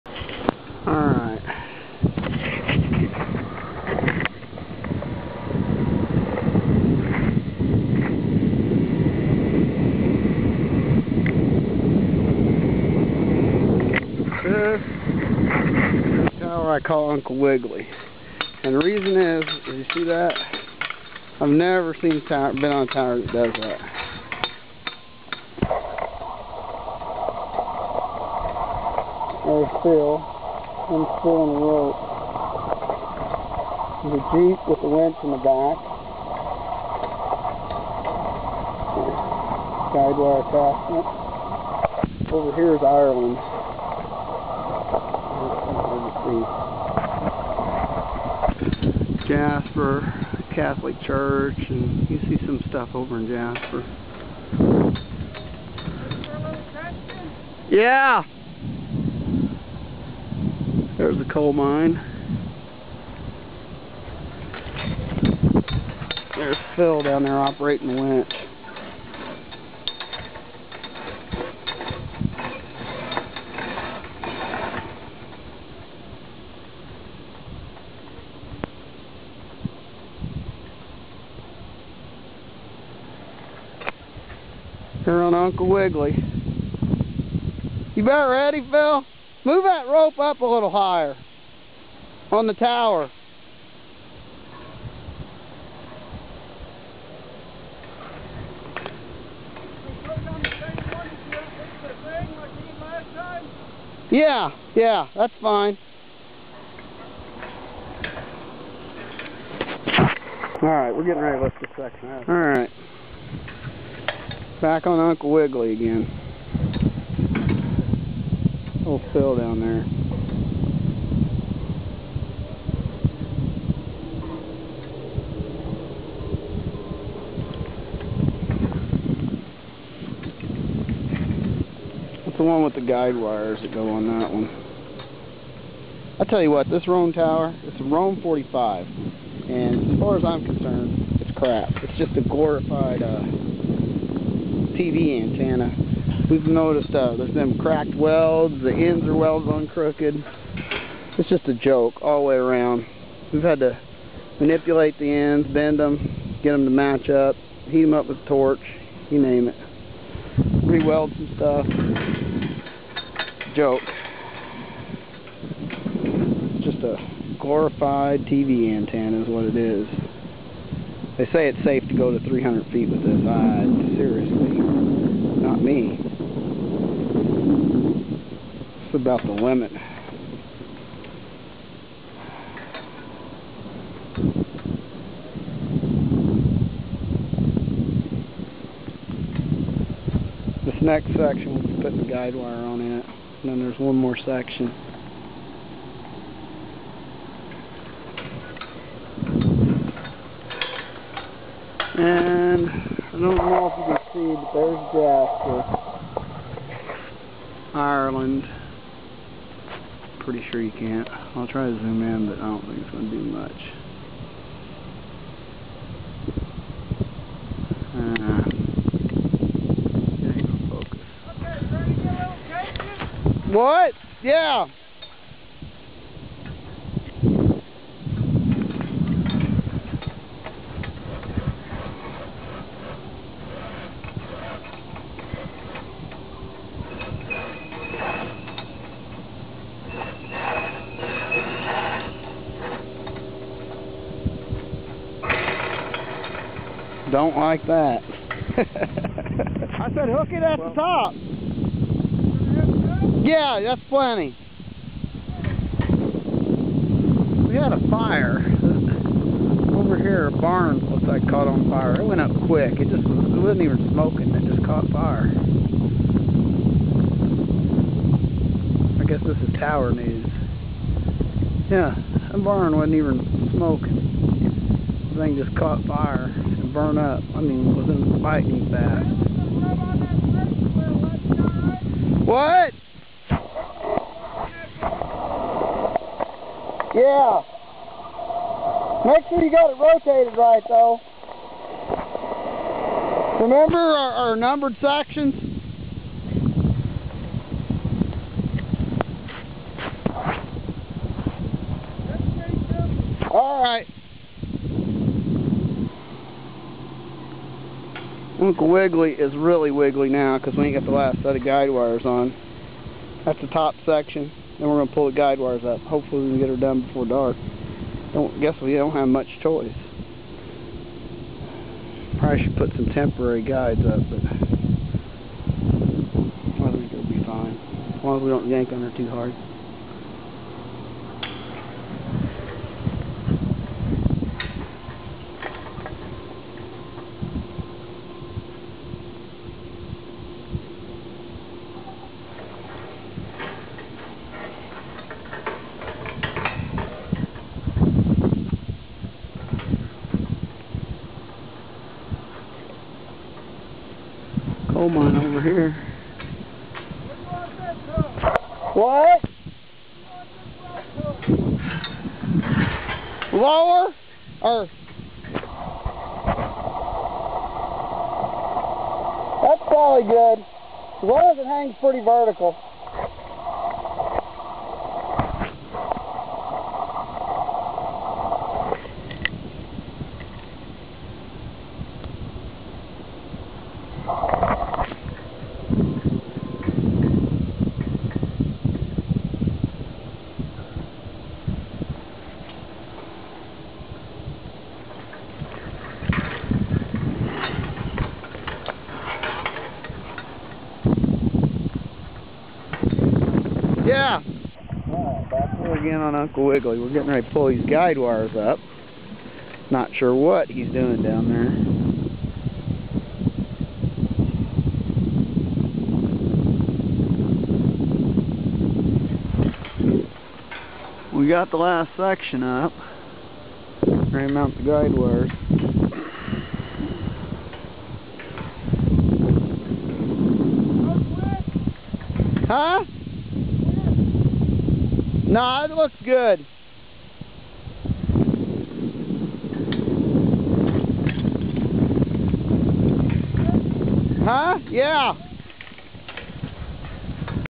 ROHN 45G Guyed Tower Series
Installing the last 3 of 15 sections and pulling up last set of guy wires of a ROHN 45G Guyed Tower Series.